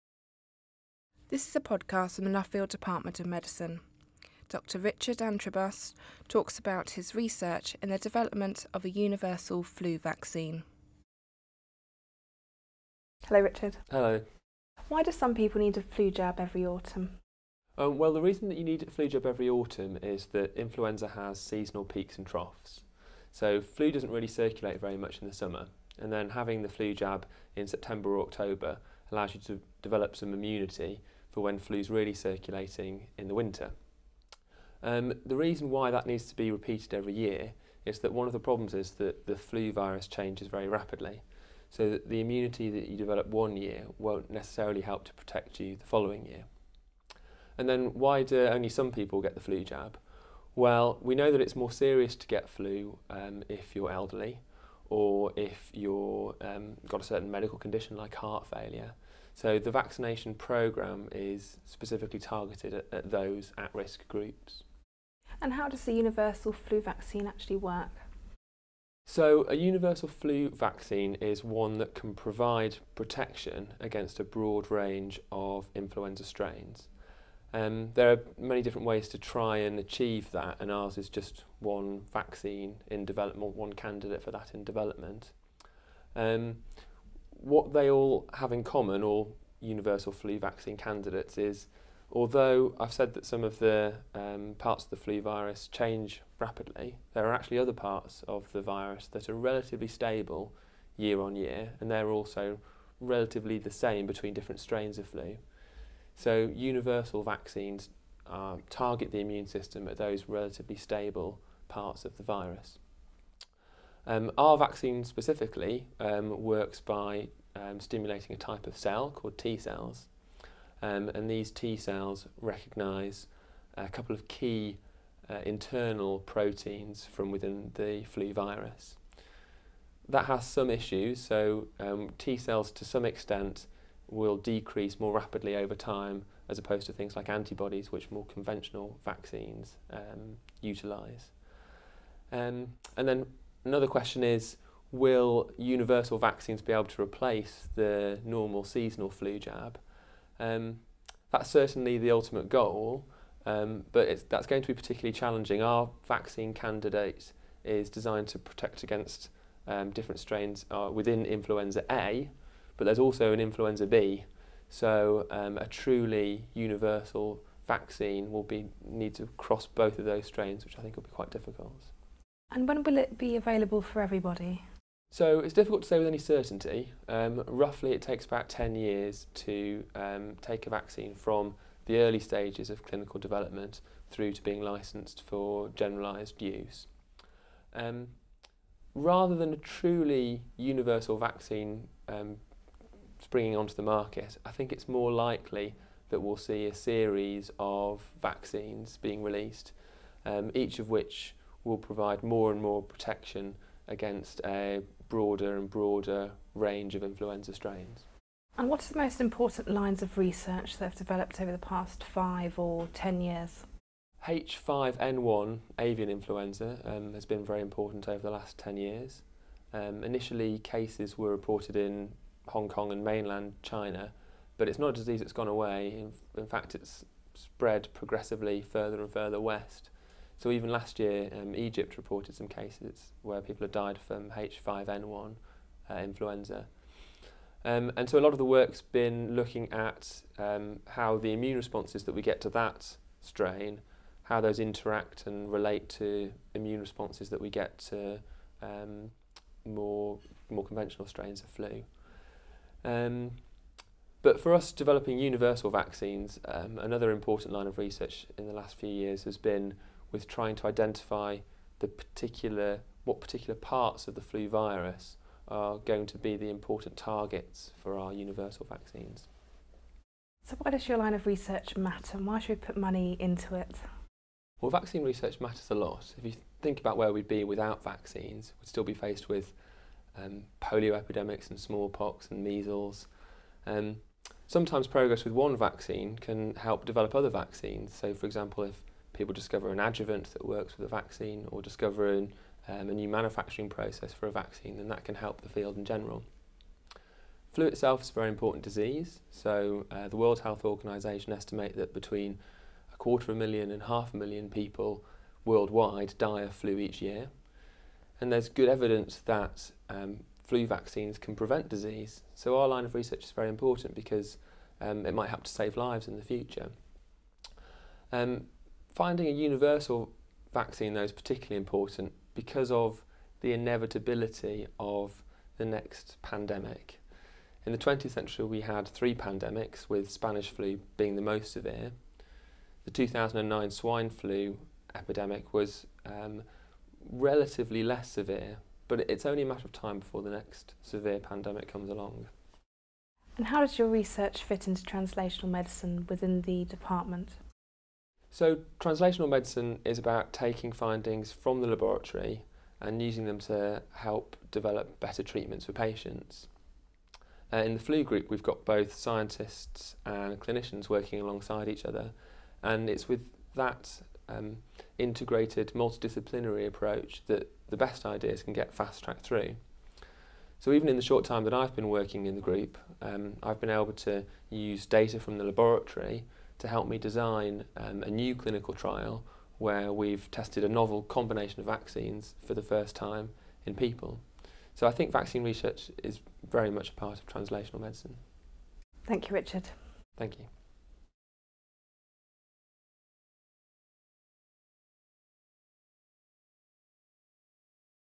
Richard Antrobus: Universal Flu Vaccine
Immunization programmes

Dr Richard Antrobus has been conducting a Phase I study of a Universal Vaccine for Influenza. Progress so far indicates that this approach induces a potent T cell response, and Dr Antrobus is currently assessing whether this response is equally potent in the elderly -- one of the 'at risk' groups of the Influenza virus.

Influenza vaccine

A Universal Flu Vaccine would protect against a wide range of strains of the virus. Universal vaccines target the parts of the virus that stay relatively stable and are the same between different strains of flu. The ultimate goal is to produce a vaccine that will eventually replace the normal seasonal flu jab.